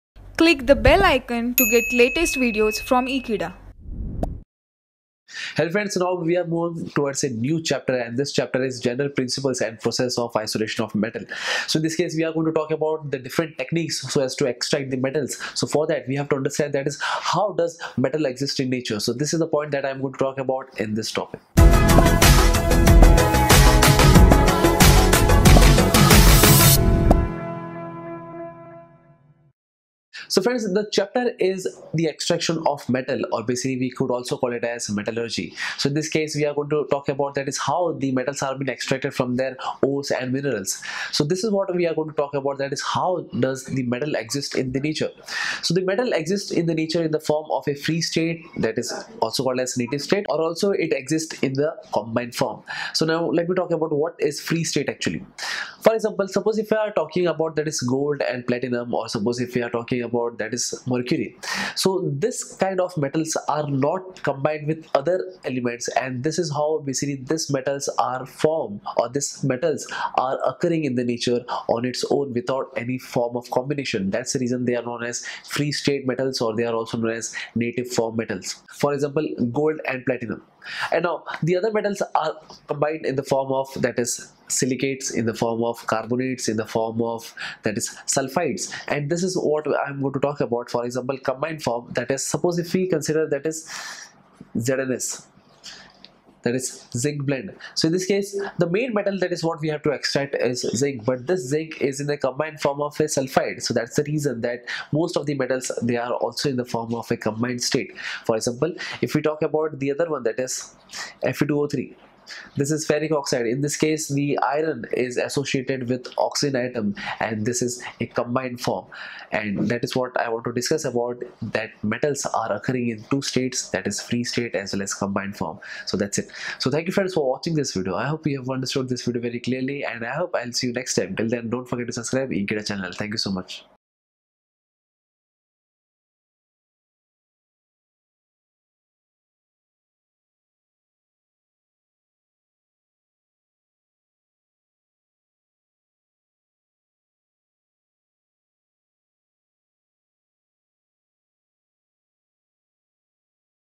Occurrence of Metal in Nature - Metallurgy - Chemistry Class 12
Occurrence of Metal in Nature Video Lecture from Metallurgy Chapter of Chemistry Class 12 for HSC, IIT JEE, CBSE & NEET.

Happy Learning : )